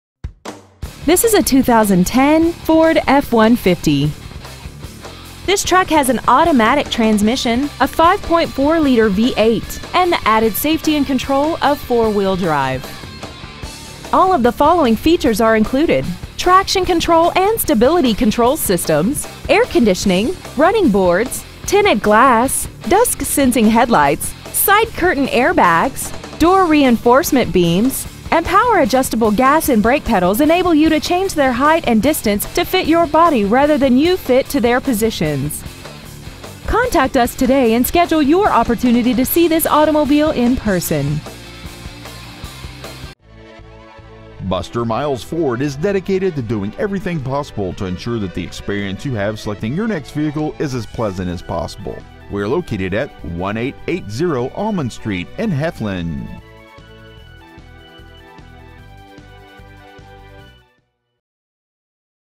2010 FORD F-150 Heflin AL
We have been honored to serve the Heflin AL area , we promise that your experience at our dealership will exceed your expectations !

 Year : 2010
 Make : FORD
 Model : F-150
 Engine : 5.4L V8 24V MPFI SOHC FLEXIBLE FUEL
 Trans . : 6-SPEED AUTOMATIC
 Exterior : BLACK
 Miles : 99,418
 Interior : TAN
 Stock : 11700A